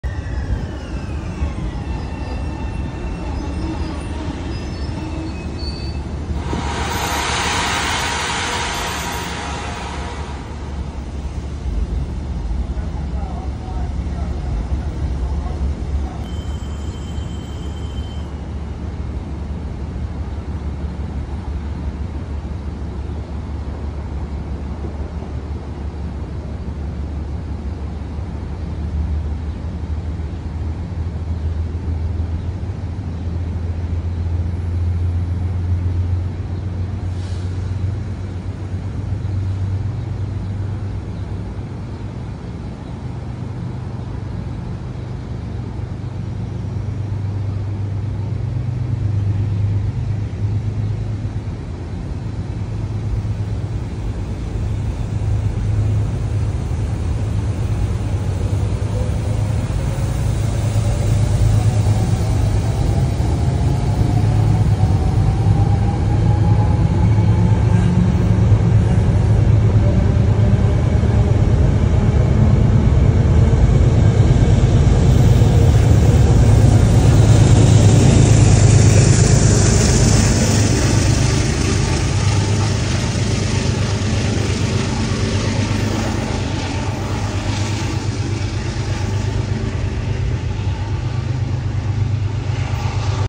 MNCR 6101 Bombardier Shoreliner Cab as the lead car | 125th Harlem Metronorth station
100% MY CONTENT • ALL VIDEOS AND PICTURES TAKEN BY ME •